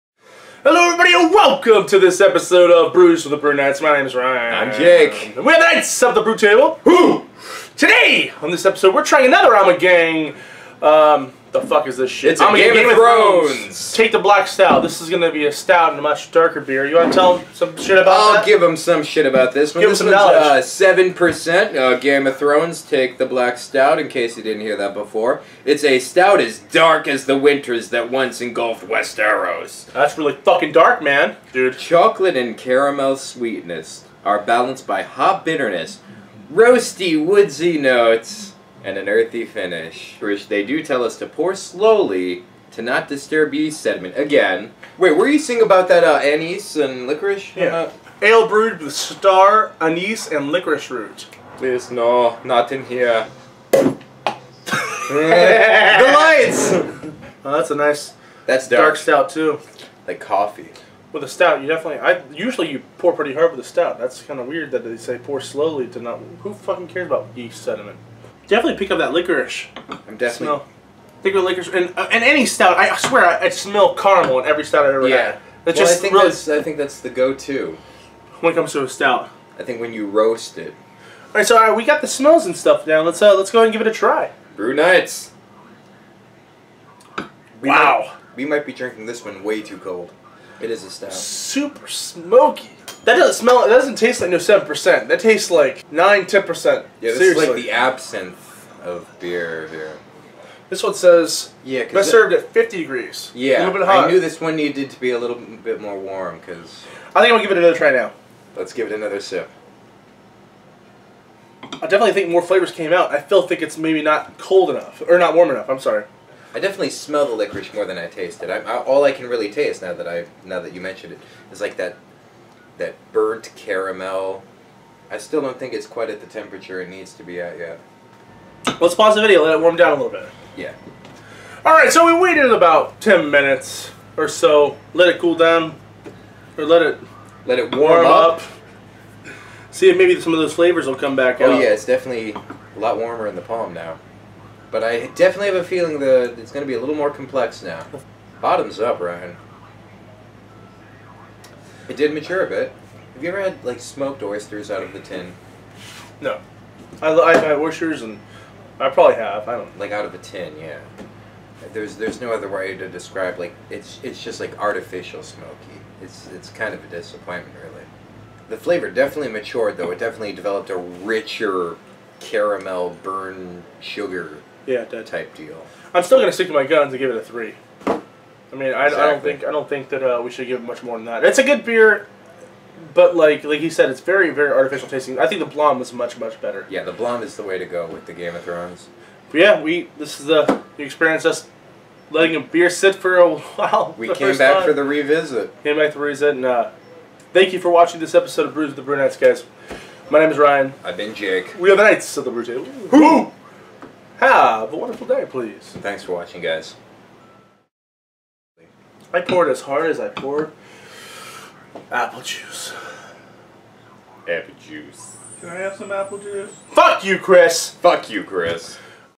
Brews with the Brew Knights, Ep. 13: Ommegang's Take the Black Stout
Another Ommegang beer from the line of Game of Throne's products. Here is the Take the Black Stout. A Belgian Dark Ale holding a hefty 7% abv. What will the guys think of this smoky sensation?